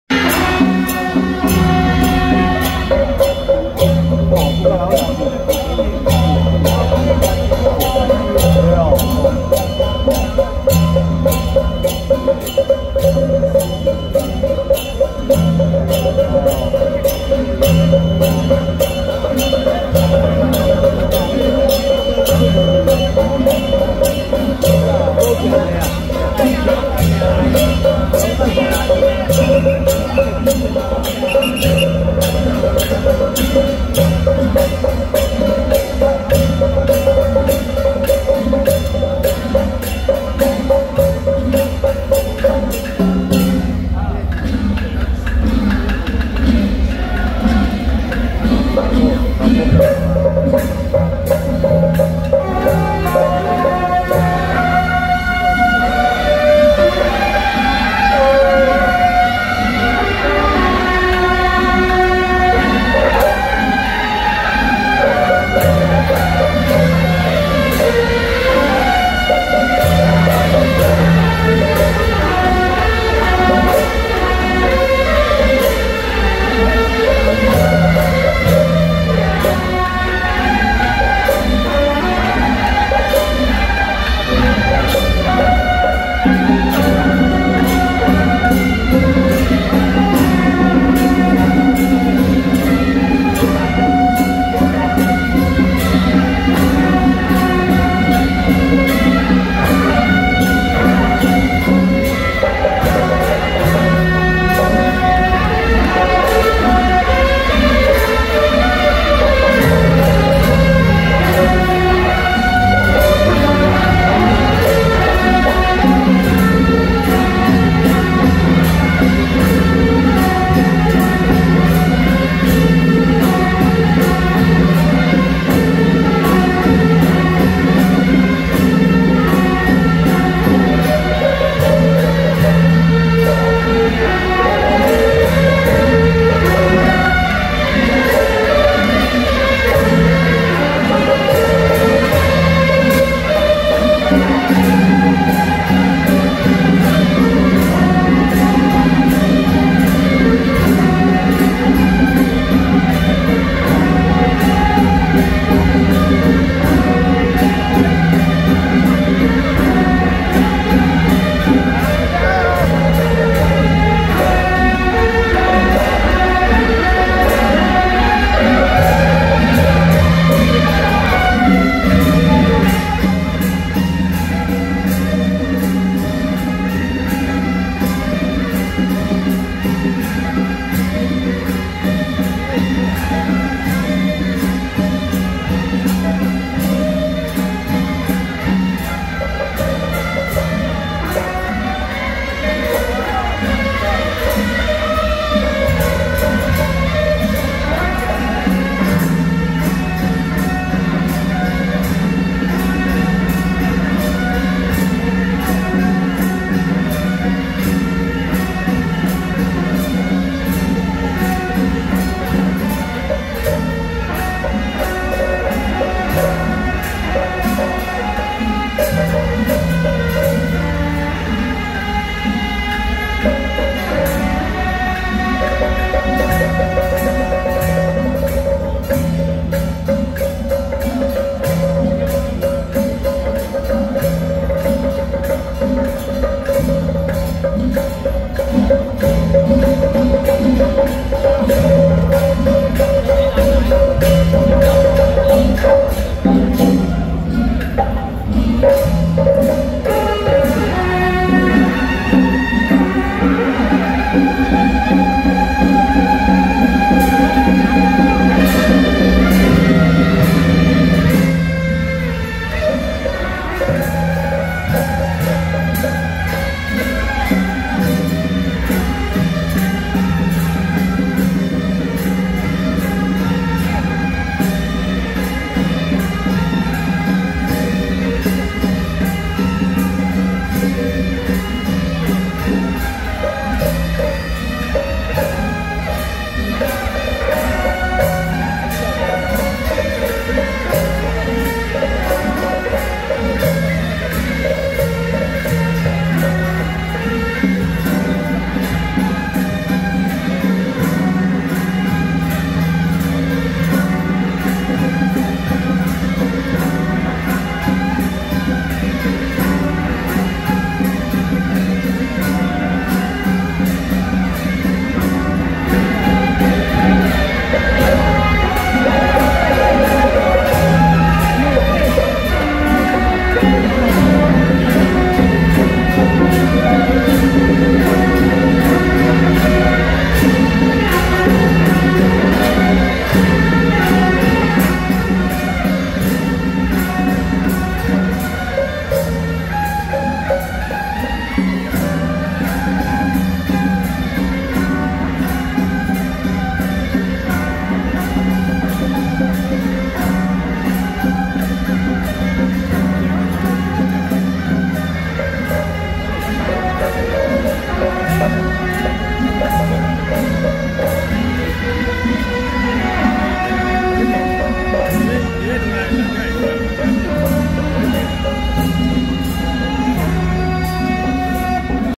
【敲王爺＆桃園菓林太子宮-中壇元帥 聖駕(竹圍張家轎班團)】~112大園仁壽宮慶祝感天大帝來台186週年暨重建聖殿安座紀念遶境
敲王爺＆桃園菓林太子宮-中壇元帥 聖駕(竹圍張家轎班團)